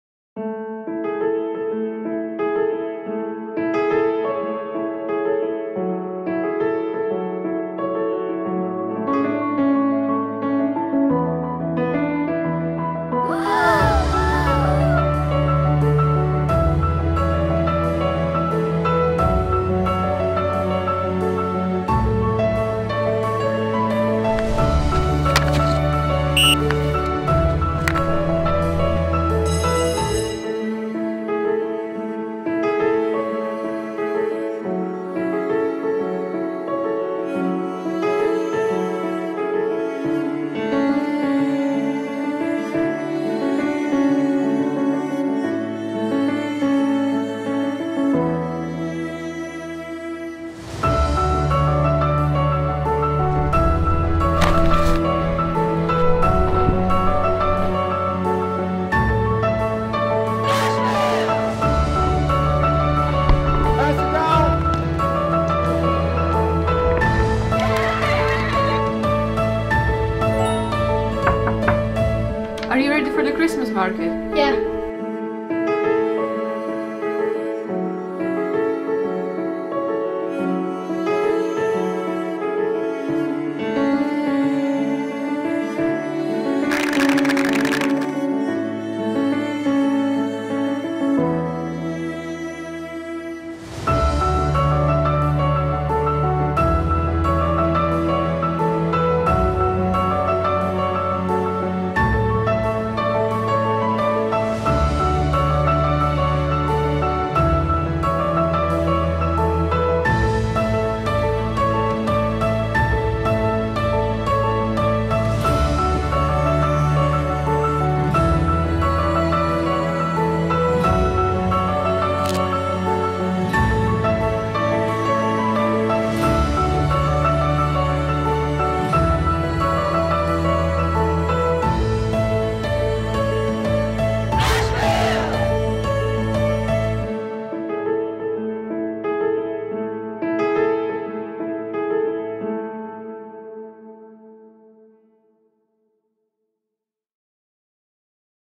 There's no place like Ashville
Ashville College is a leading independent co-educational day and boarding school for children aged 2-18 in Harrogate, North Yorkshire.